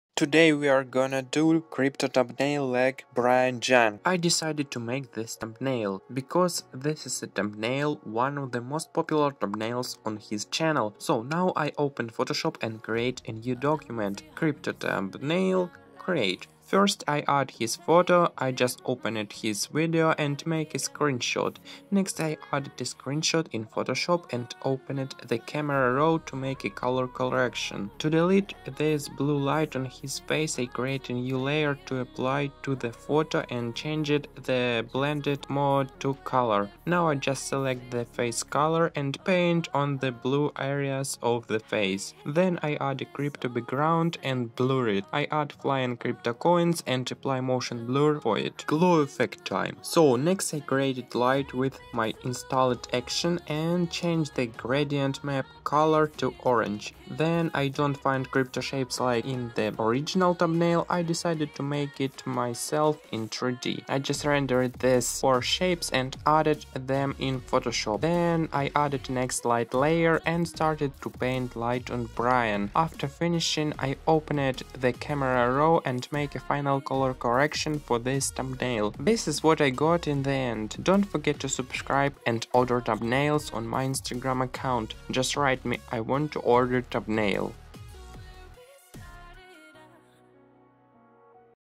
How To Make a Crypto Thumbnail Like Brian Jung | YouTube Thumbnail Agency
Hi! In this video, I will show you how to make a crypto thumbnail like Brian Jung in Photoshop. Brian Jung is a very famous crypto YouTuber because his thumbnails are unique. So today I will repeat his thumbnail. Enjoy!

//about
My name's Yaroslav and I design thumbnails. I usually do beautiful things. If you have any ideas, please let me know.

Save this video for later ❤